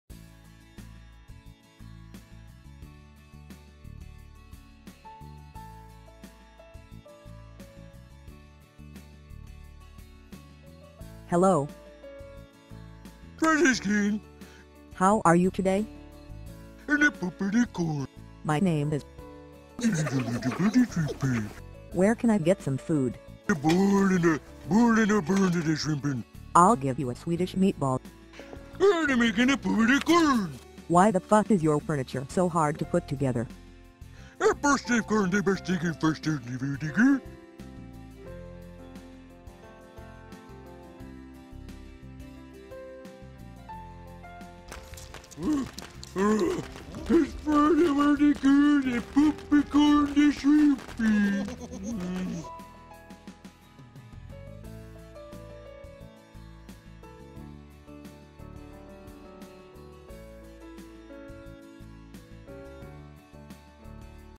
Learn Simple Swedish Phrases in a Minute (Funny)
- Movies, Music, Girls, Gear and Beer

Warning:This video will not actually teach you Swedish.  If you repeat these phrases to a Swedish person, MMGGB.com and its affiliates are not responsible for any repercussions.. including but not limited to: Banishment, being pelted with Swedish Meatballs, Genital Warts and being forced to set up a living room's worth of furniture from Ikea.